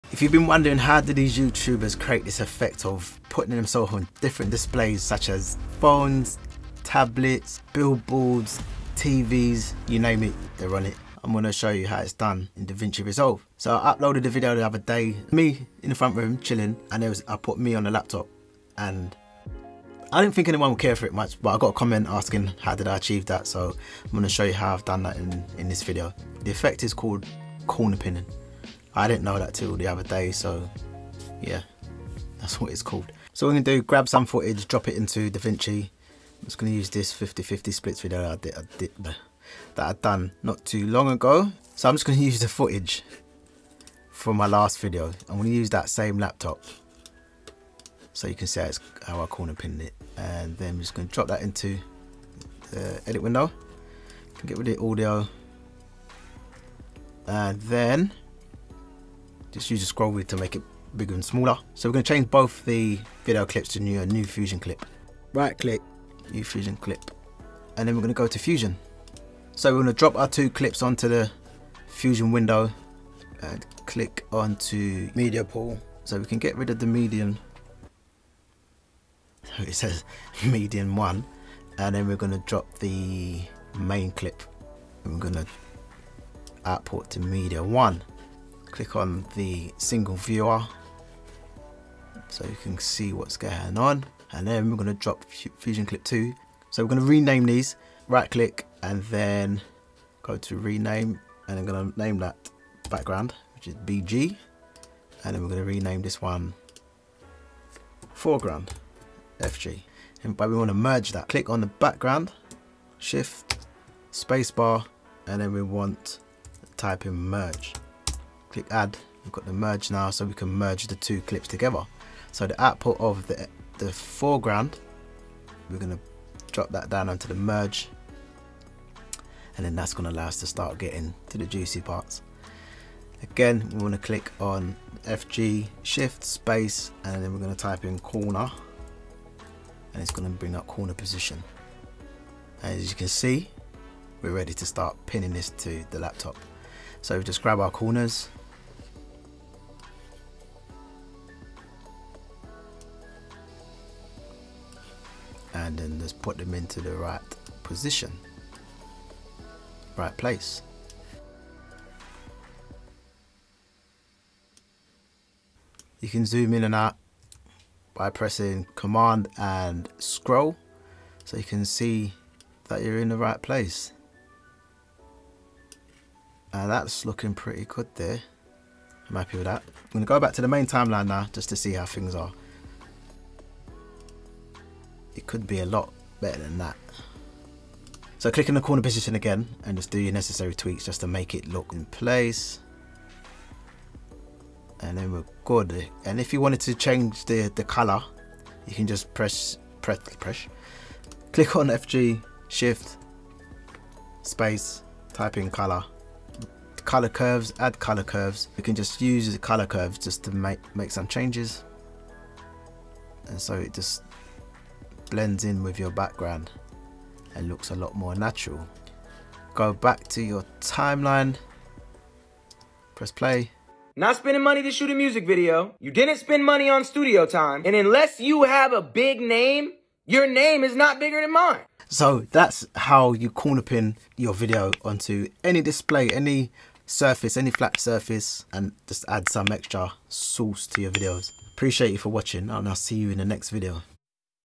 Screen replacement: Pin your video or pic to anything in DaVinci Resolve
DaVinci Resolve Tutorial: Screen Replacement (FAST Corner pinning)
Make your videos pop with this editing trick in Davinci Resolve. You can use this video effect to add some sauce to your content.

It's simpe and easy to do, once you follow the method that I show you in this video.

This video will cover:
✅ The step-by-step process of corner pining your videos to another display
✅ How to use Fusion to create this effect (Basic)

C O N N E C T  O N  S O C I A L   M E D I A:

M U S I C   P R O D U C T I O N   S E R V I C E S:

S H O P  M U S I C  T E C H  G E A R